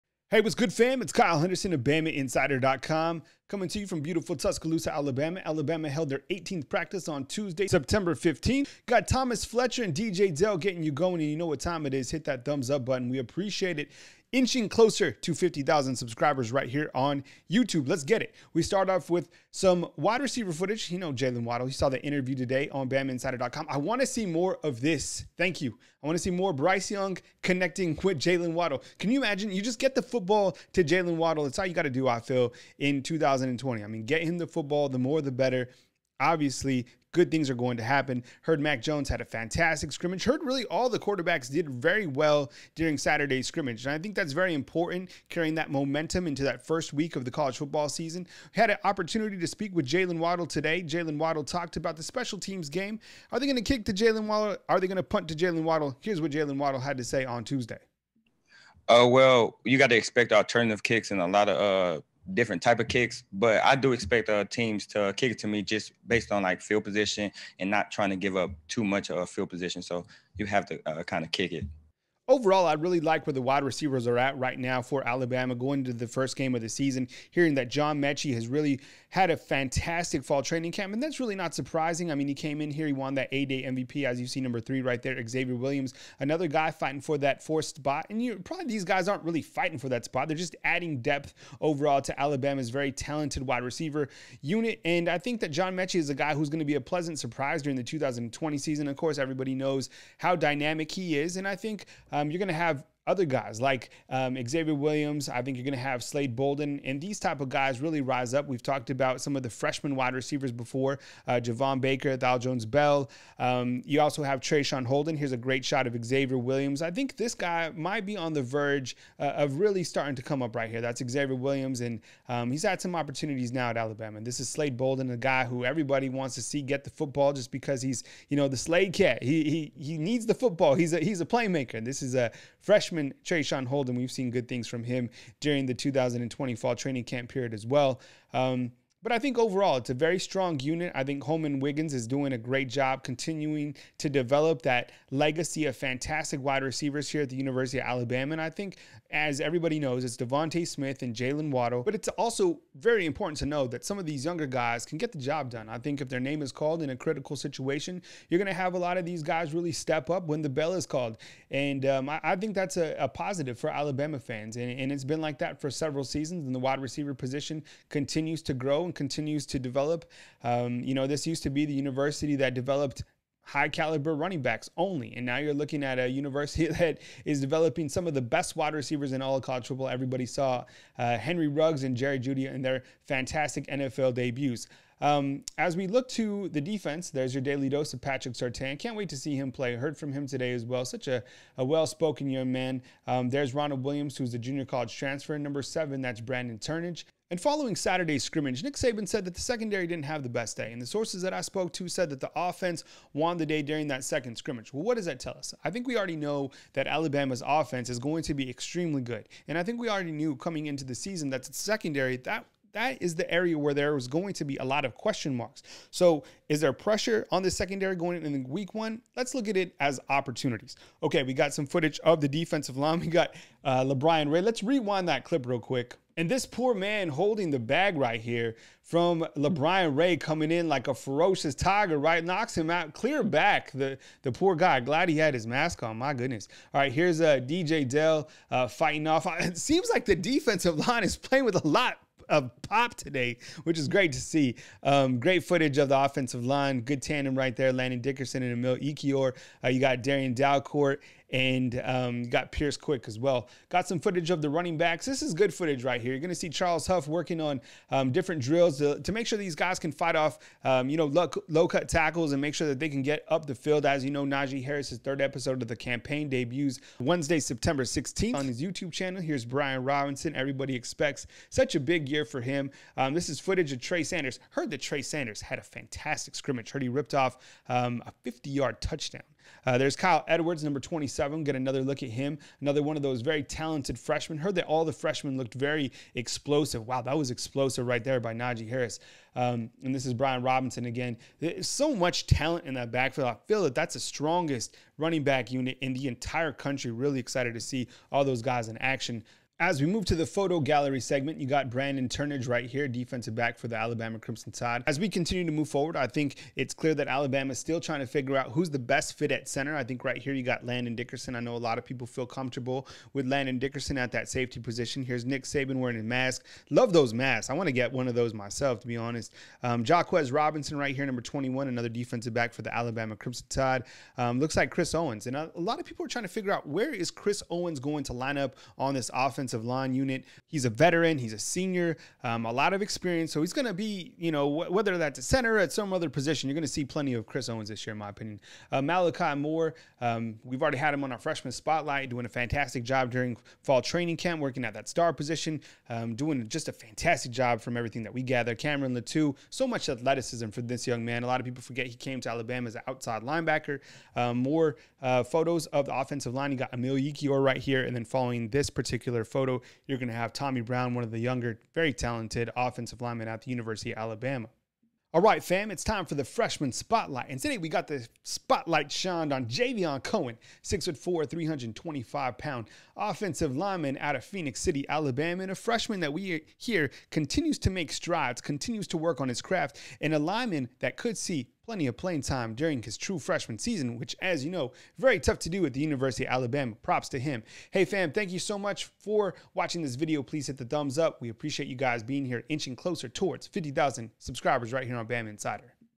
Watch Alabama Football practice: Najee Harris, Bryce Young, Jaylen Waddle, LaBryan Ray, DJ Dale
Watch Alabama Football practice: Mac Jones, Bryce Young, Jaylen Waddle, LaBryan Ray, DJ Dale on September 15